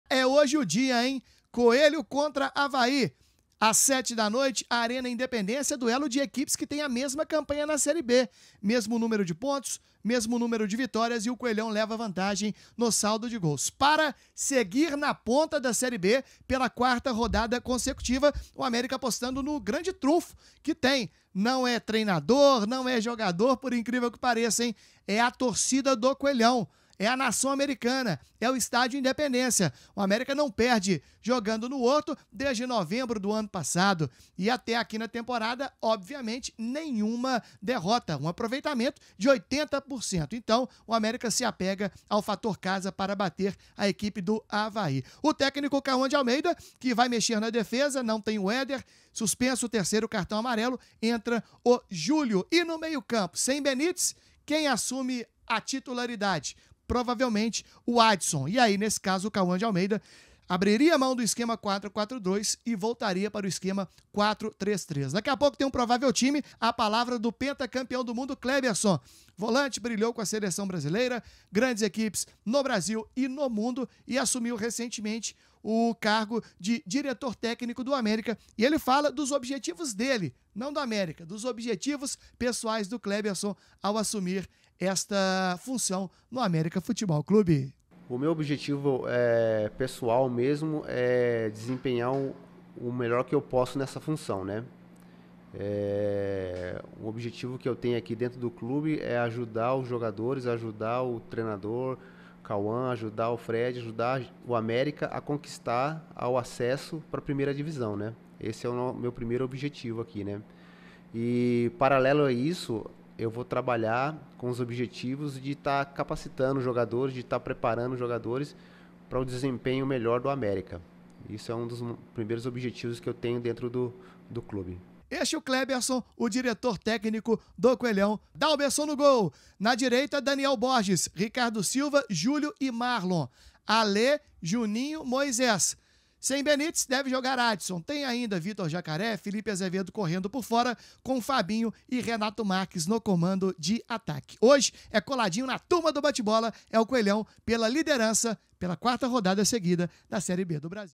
DIA DE AMÉRICA X AVAÍ NO HORTO PARA DEFENDER A INVENCIBILIDADE EM CASA | PRIMEIRAS DO AMÉRICA
O América recebe o Avaí nesta terça-feira (25) defendendo a marca de ainda não ter perdido em casa no Brasileirão da Série B. Confira as primeiras informações com o repórter Fabrício Calazans.